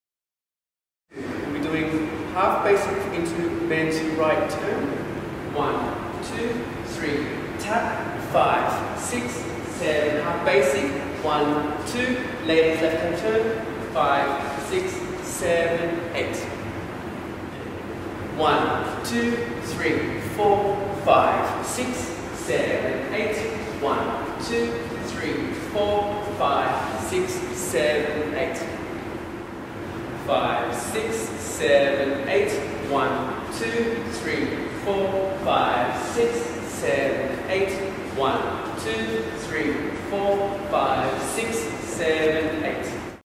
Bachata Beginners Combo 3
Bachata combination 3:
- Half basic
- Mens right turn (pass hand behind the back)
- Half basic
- Ladies left turn

Instructors: Chris & Quyen